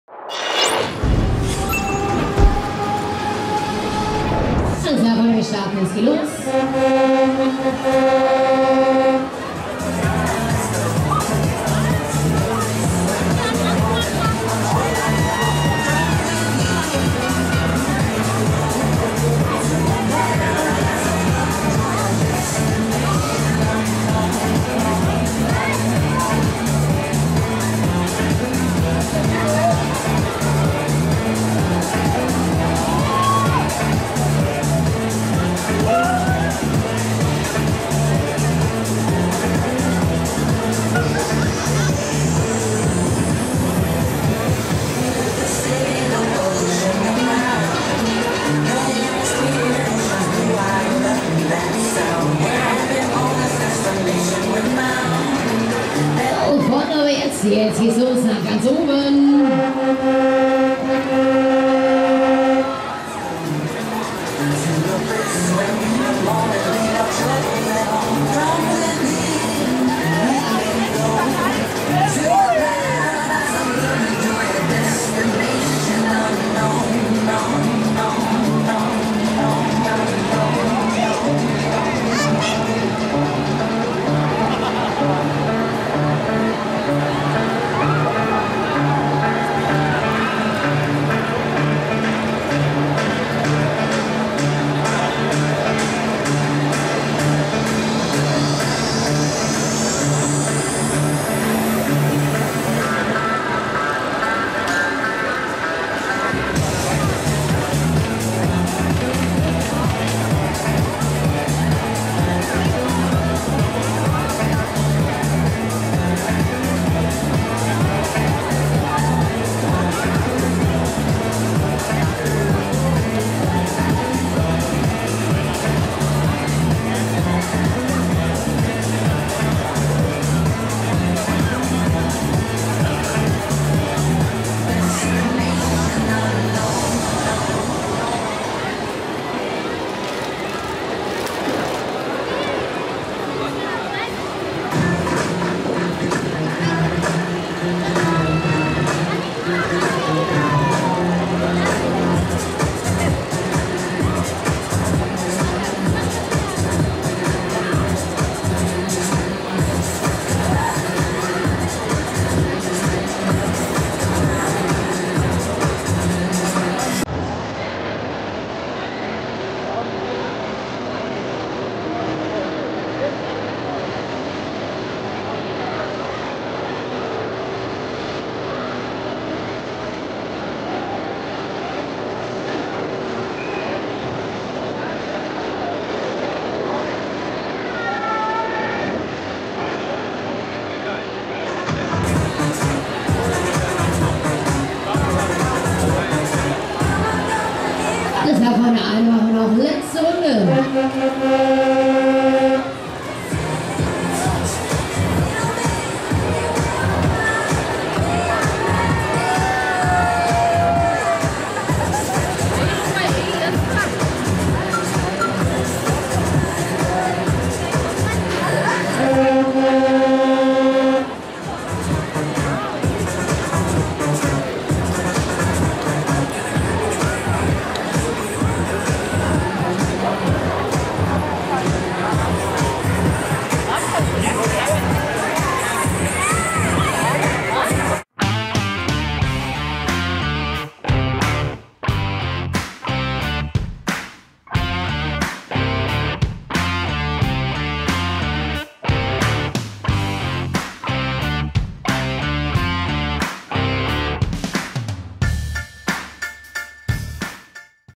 Wellenflug Distel Offride Nürnberger Herbstvolksfest 2024 [FULL HD]
In diesem Video seht ihr ein Offride des Wellenflug von Distel auf dem Nürnberger Herbstvolksfest 2024.